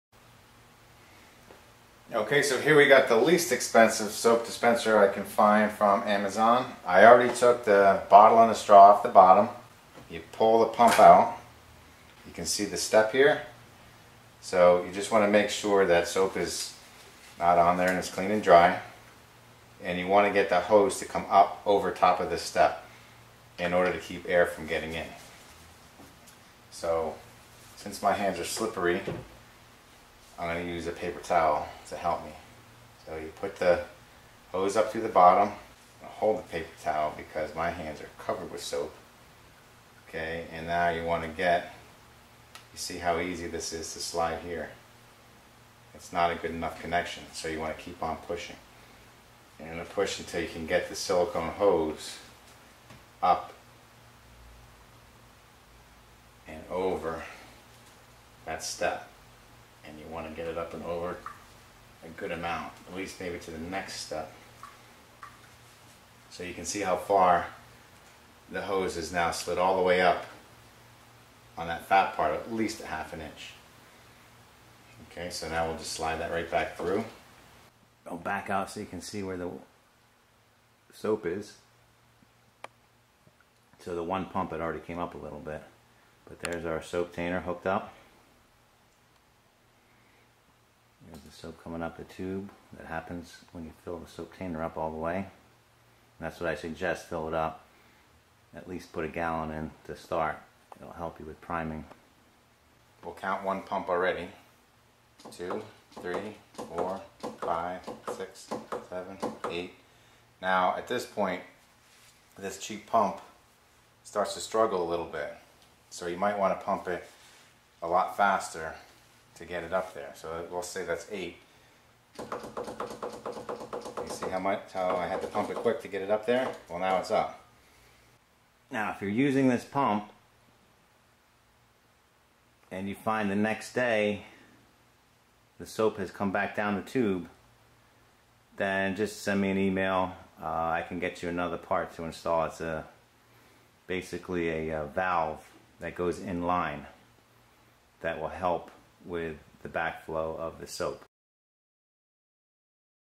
Connecting Soaptainer to Ultra Faucet's Soap Dispenser
If you have an Ultra Faucet soap dispenser and would like to connect Soaptainer to it please watch this helpful video!